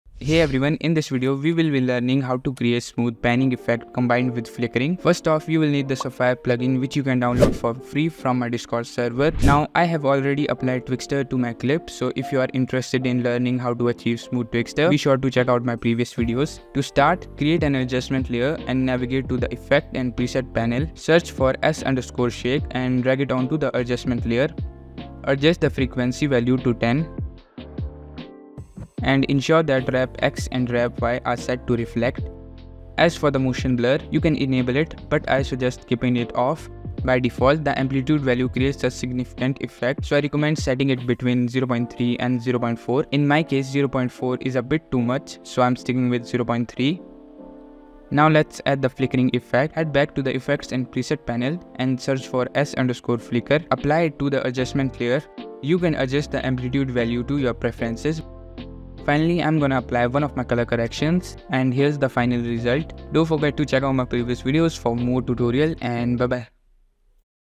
Create Smooth Panning & Flicker Effects in After Effects | After Effects Tutorial
In this tutorial, learn how to add a touch of magic to your videos with smooth panning and flicker effects in After Effects. Elevate your content creation game and captivate your audience with these dynamic visual techniques. Watch now to master the art of seamless motion and vibrant flicker in your videos!